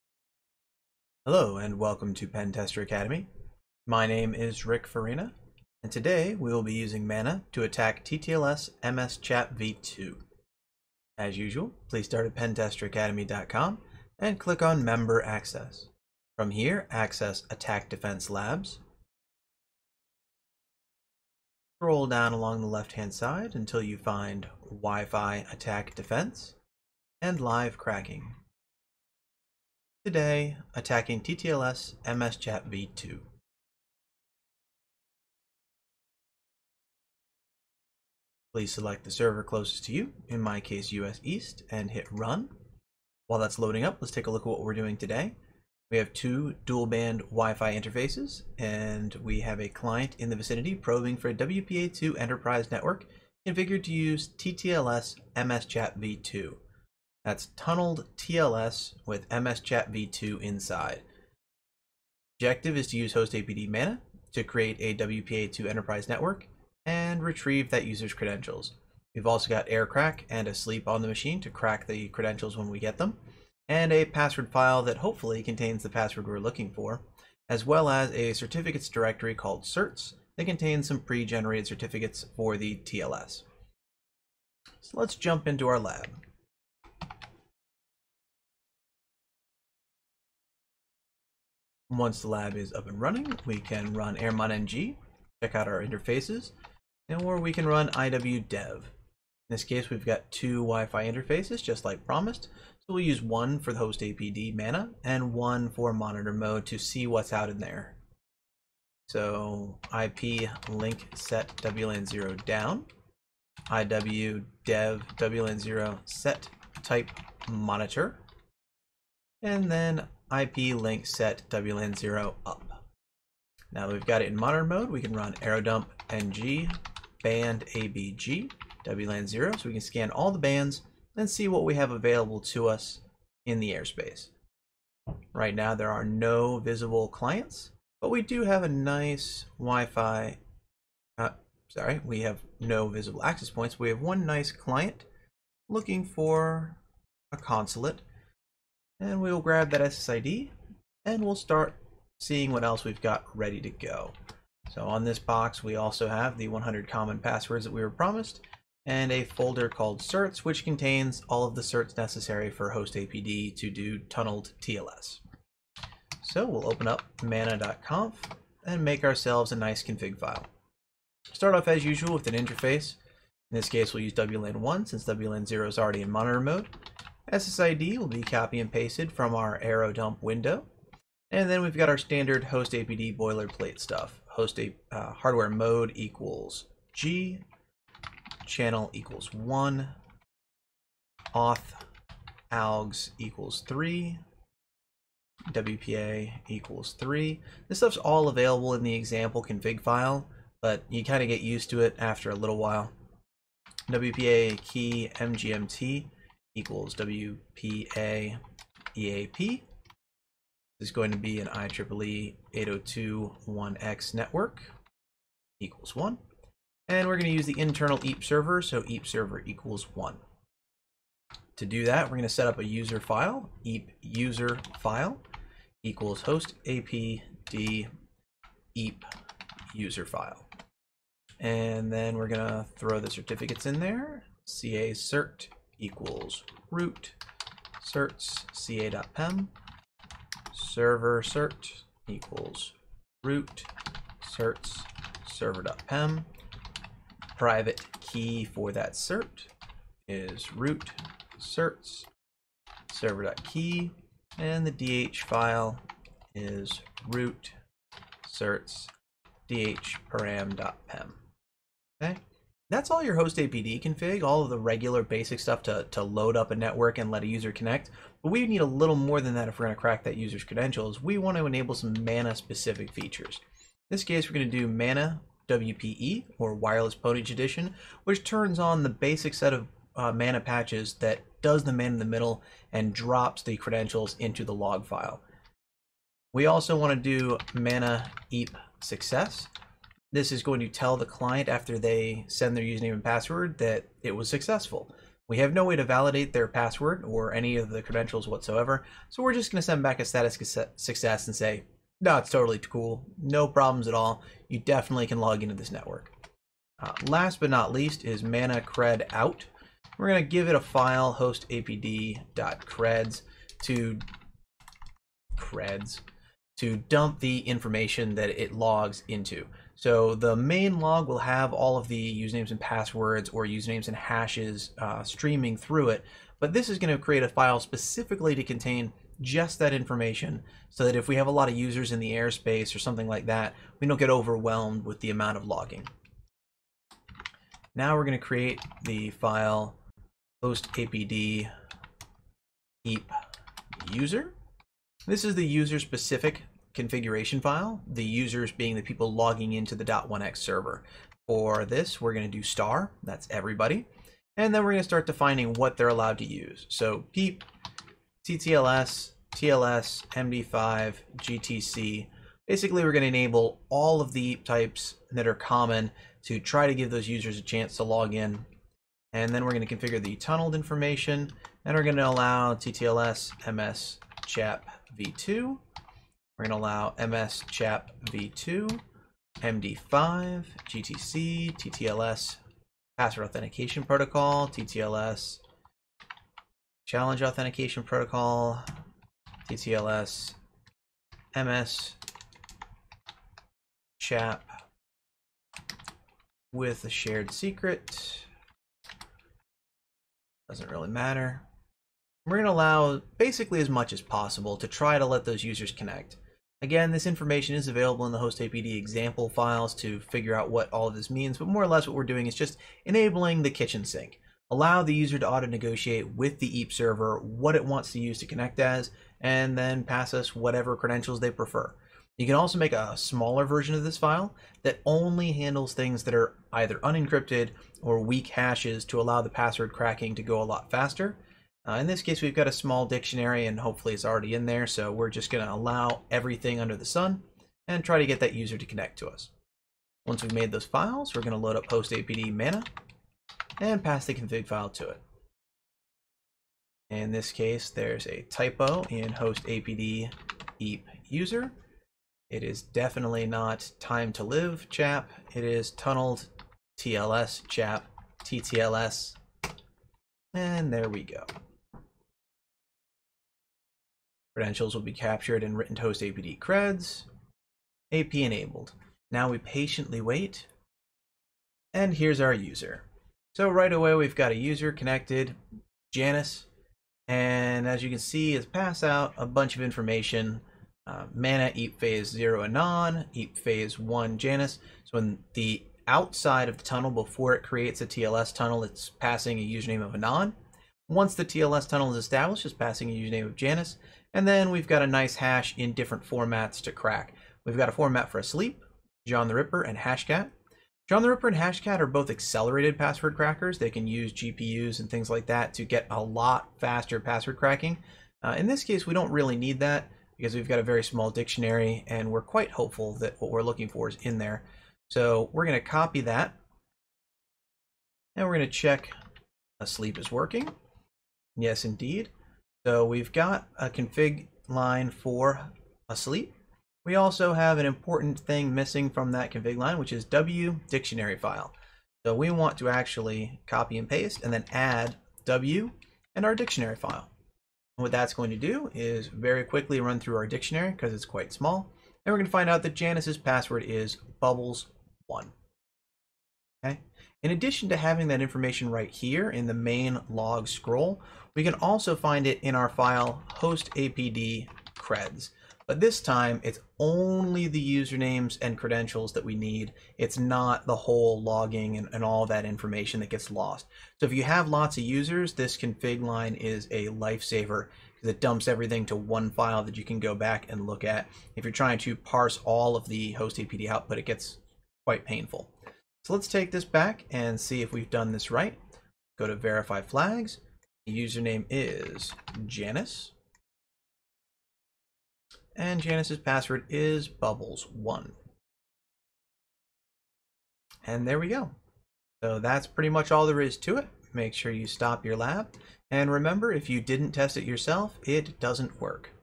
WiFi Security: Mana: Attacking TTLS-MSCHAPv2
WiFi Security lab walkthrough: See how TTLS-MSCHAPv2 can be attacked by creating a honeypot with Hostapd-mana!

📜Certifications and exam attempts for both aspiring and seasoned red teamers